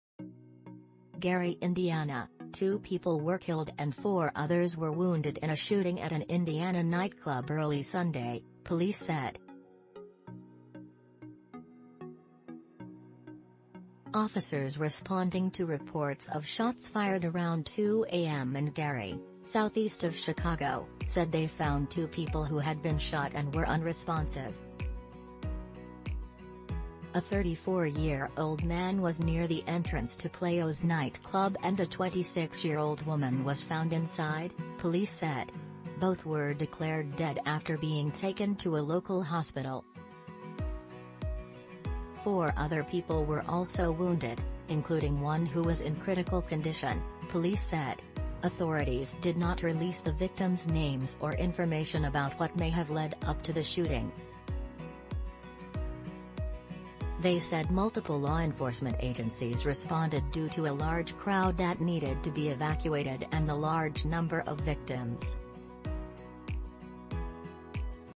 2 people were killed in a shooting at a nightclub in Gary Ind
GARY, Ind. — Two people were killed and four others were wounded in a shooting at an Indiana nightclub early Sunday, police said. Officers responding to reports of shots fired around 2 a.m. in Gary, southeast of Chicago, said they found two people who had been shot and were unresponsive. A 34-year-old man was near the entrance to Playo's NightClub and a 26-year-old woman was found inside, police said. Both were declared dead after being taken to a local hospital. Four other people were also wounded, including one who was in critical condition, police said. Authorities did not release the victims' names or information about what may have led up to the shooting. They said multiple law enforcement agencies responded due to a large crowd that needed to be evacuated and the large number of victims.